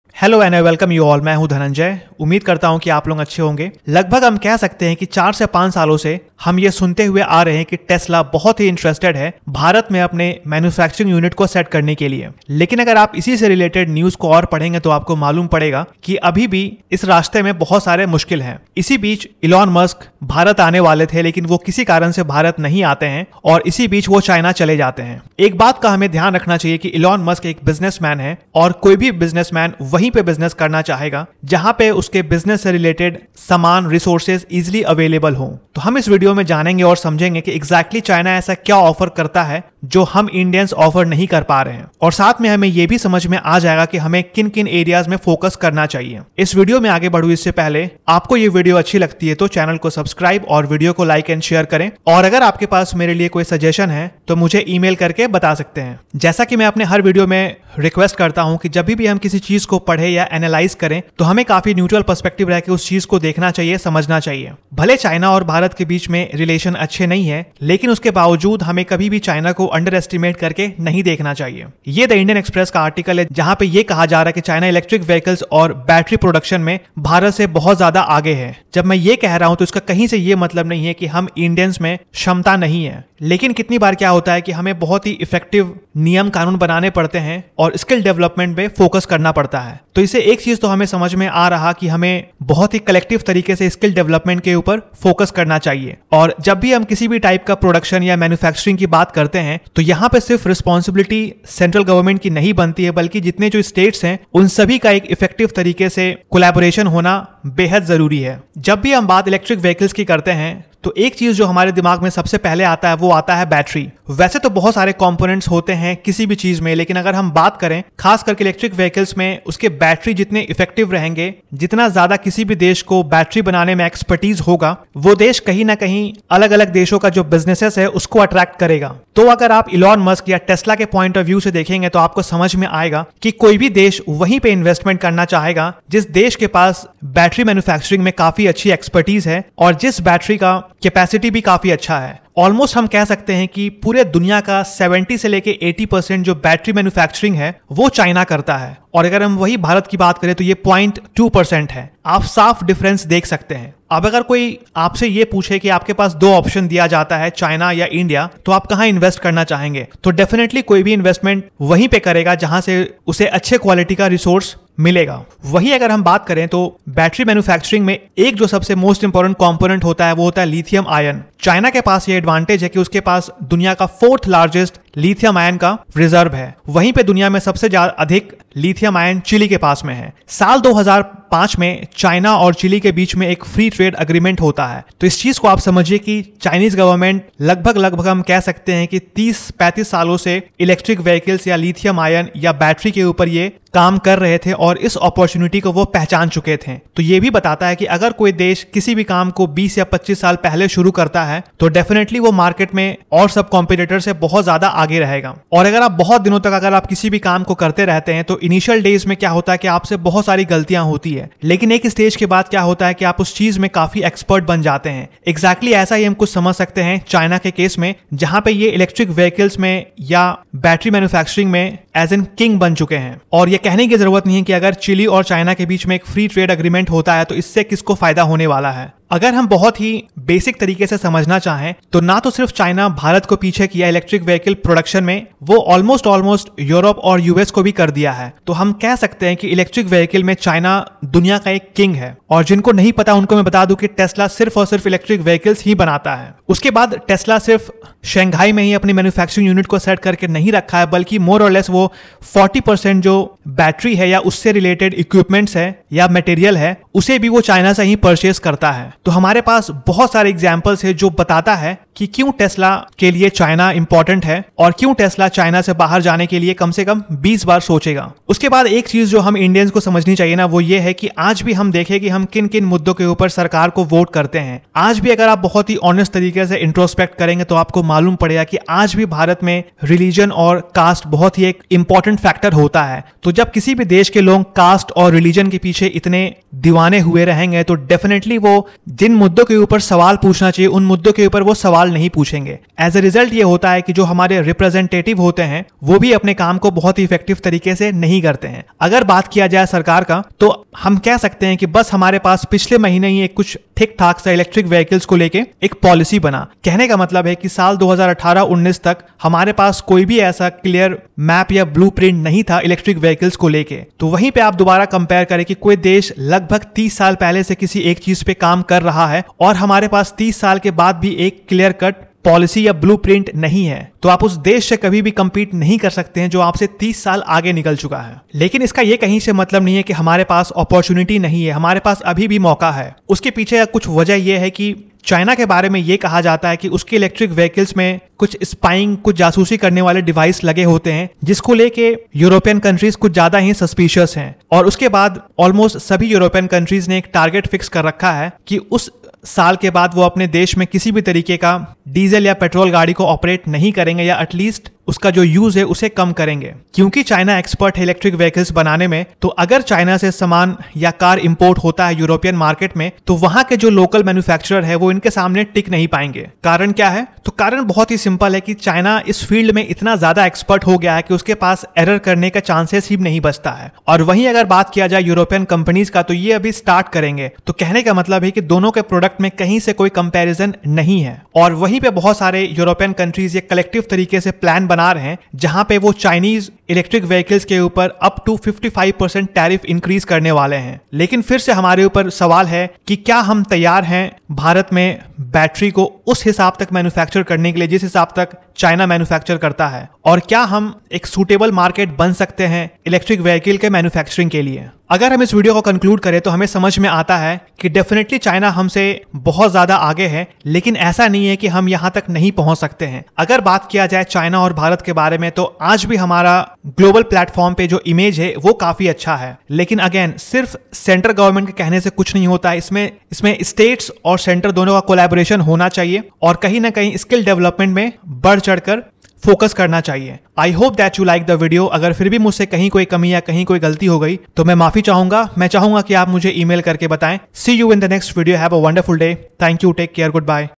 जानिए Tesla China पर कितनी ज्यादा Dependent है?
More than half of all electric vehicle (EV) sales worldwide are made in China, mostly due to the country's near-monopoly status in battery production, which is essential to the creation of EVs. Reuters said that during Elon Musk's most recent trip to China, he had a meeting with Robin Zeng, the chairman of Contemporary Amperex Technology Co Limited (CATL), a significant Chinese battery manufacturer. Two-thirds of the world's batteries are produced by CATL, which also supplies Tesla and other significant automakers like Toyota Motor Corp. and Volkswagen AG.